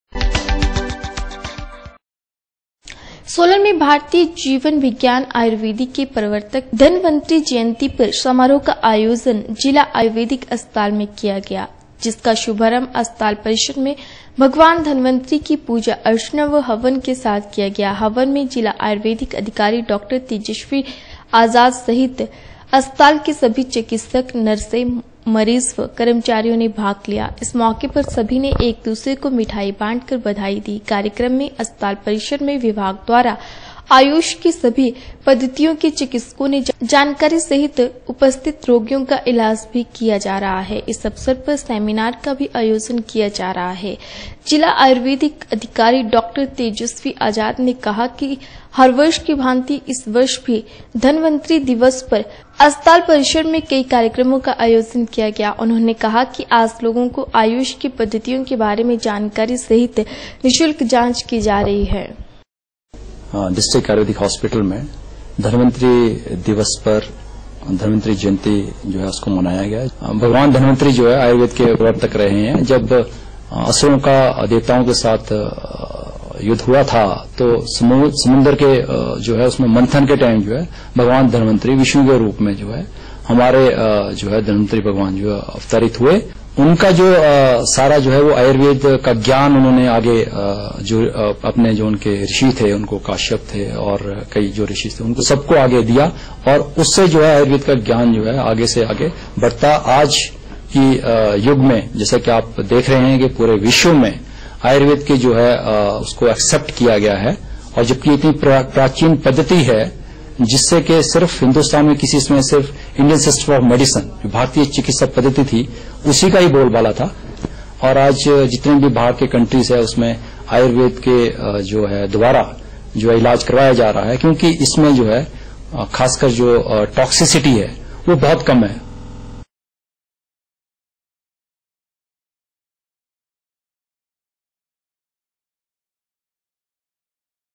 सोलन में भारतीय जीवन विज्ञान आयुर्वेदिक के प्रवर्तक भगवान धनवंतरि जयंती पर समारोह का आयोजन
सोलन में भारतीय जीवन विज्ञान आयुर्वेदिक के प्रवर्तक भगवान धनवंतरि जयंती पर समारोह का आयोजन जिला आयुर्वेदिक अस्पताल में  किया गया। जिसका शुभारम्भ अस्पताल परिसर में भगवान धन्वंतरि की पूजार्चना व हवन के साथ किया गया। हवन में जिला आयुर्वेद अधिकारी डॉ तेजस्वी आजाद सहित अस्पताल के सभी चिकित्सक एनर्सो ए मरीजो व कर्मचारियों ने भाग लिया।